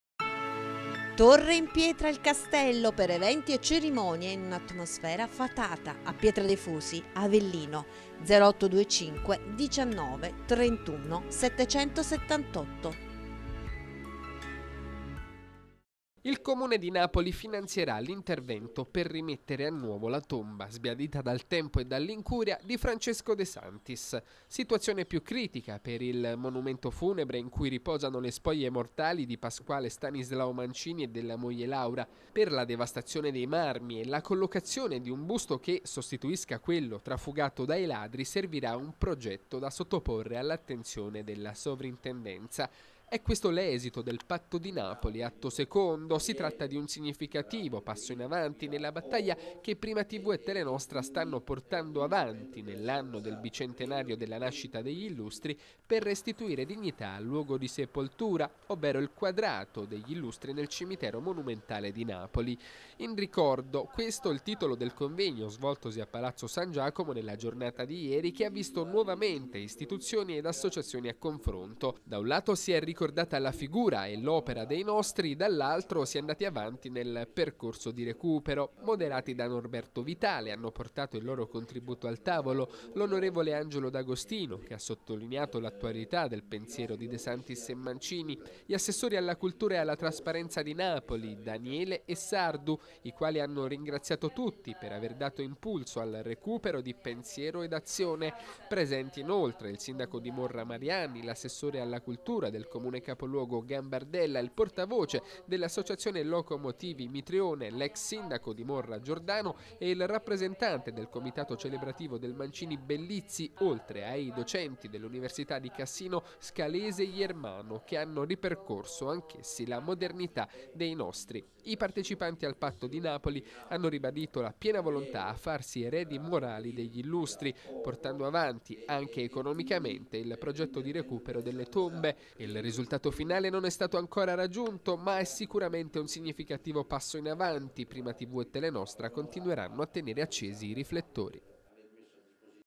Tombe illustri De Sanctis-Mancini, patto Napoli-Irpinia
Tombe illustri irpini, patto tra Napoli ed Avellino. Il Comune partenopeo si farà carico di riprendere la tomba del De Sanctis. Vinta una nostra battaglia di civiltà.